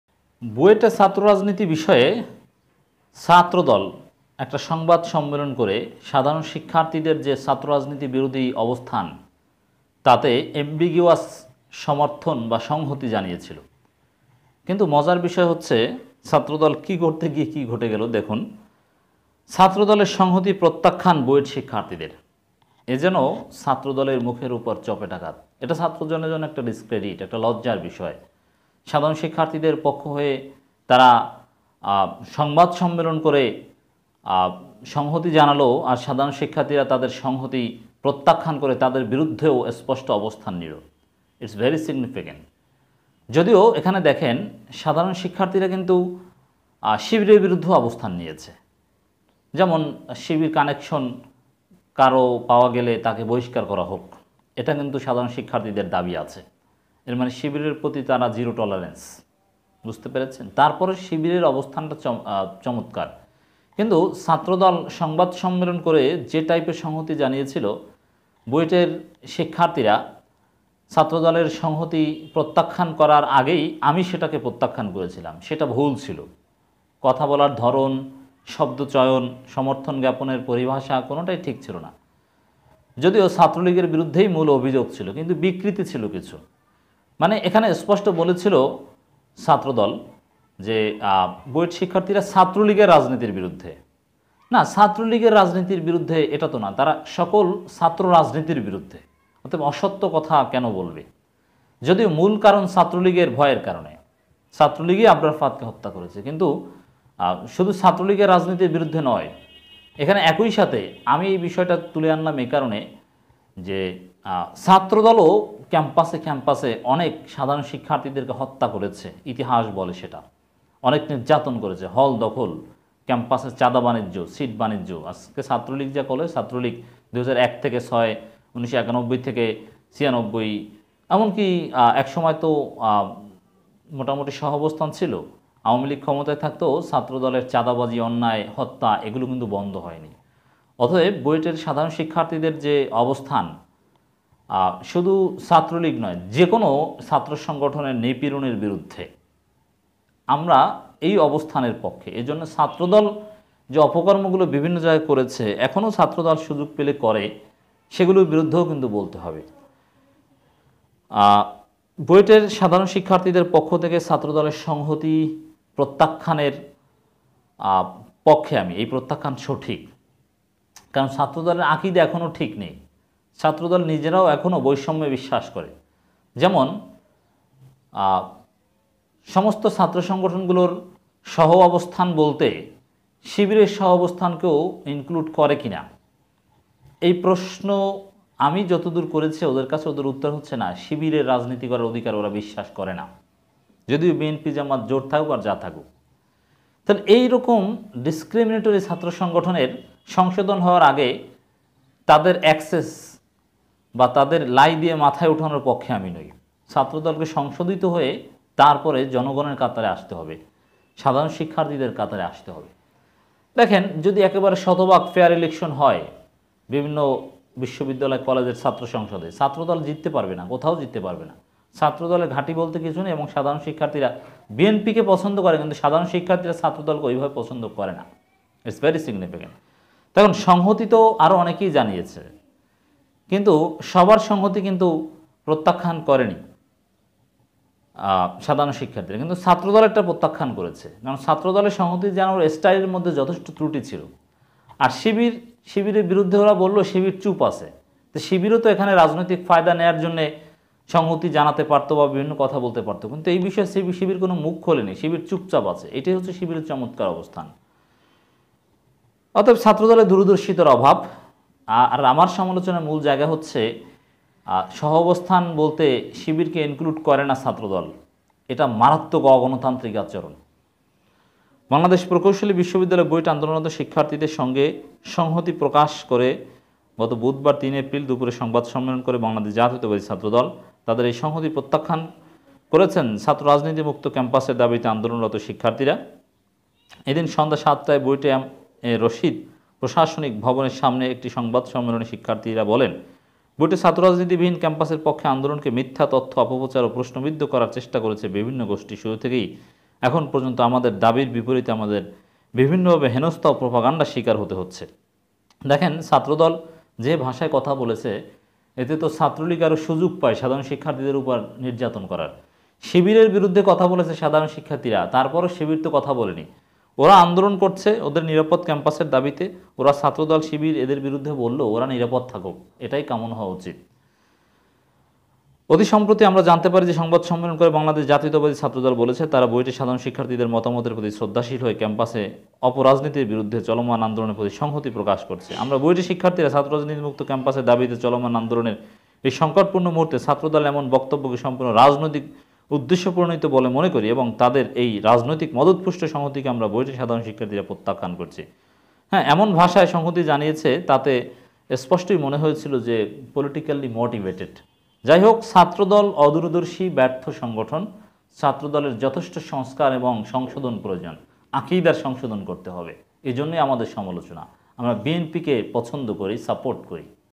বুয়েটের ঘটনা নিয়ে ছাত্রদল কী করতে গিয়ে কী হয়ে গেলো?||শিবিরের চমৎকার অবস্থান
ছাত্রদলের সংহতি প্রত্যাখ্যান করলো বুয়েটের সাধারণ শিক্ষার্থীরা!